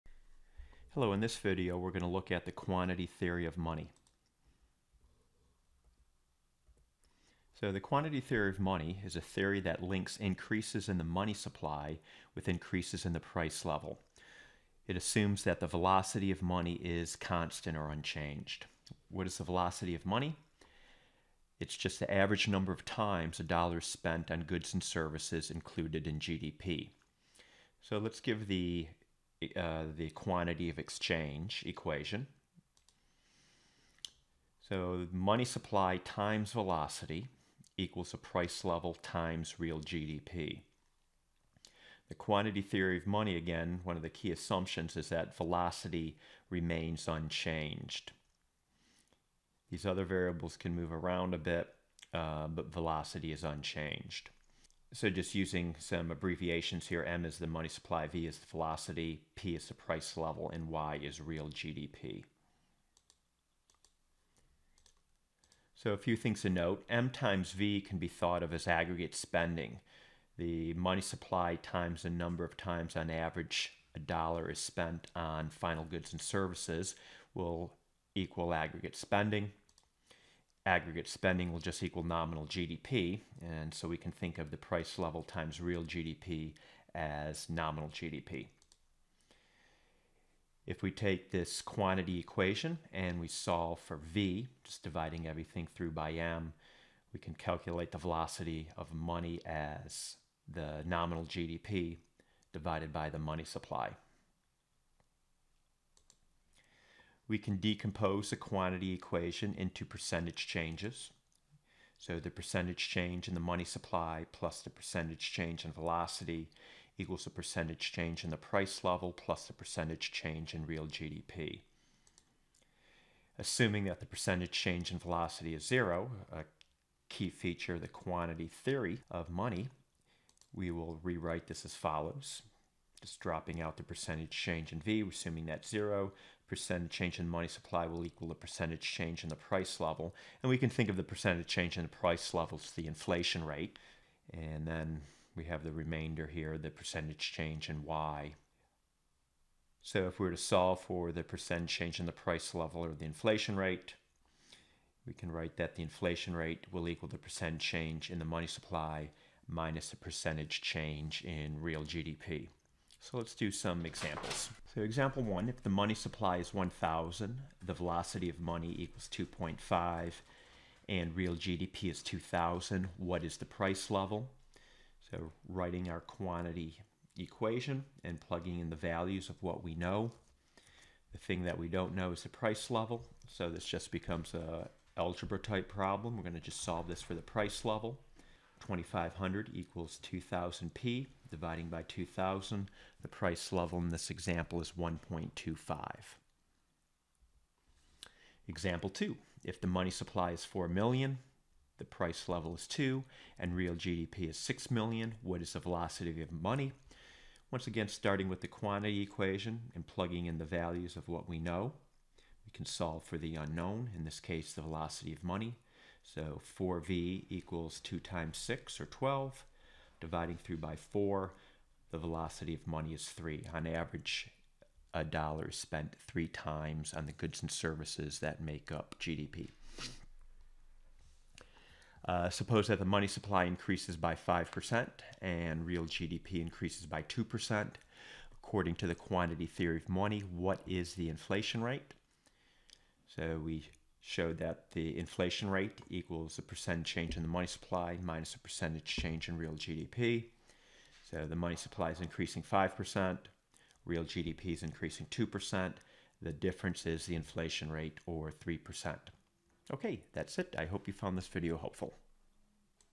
The Quantity Theory of Money
A look at the quantity theory of money and example problems.